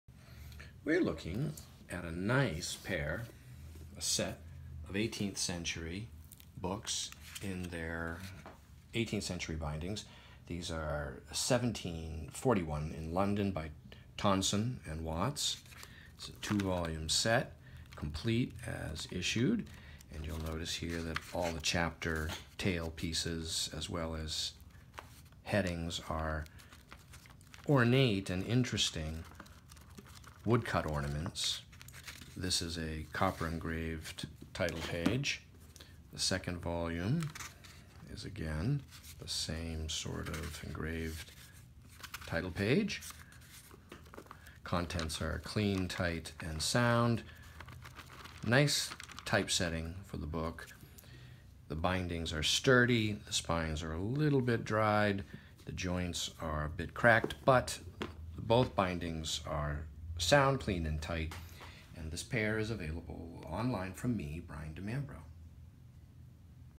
Musae Anglicanae History Anglo-Latin Poetry 1741 set 2 vols leather Tonson
In typical period English full calf leather bindings, raised bands, gilt rules, contrasting gilt title labels.

Sound, tight set, joints scuffed with surface cracking, all covers still firmly attached. Engraved pictorial title pages, profusion of small woodcut devices and ornaments throughout.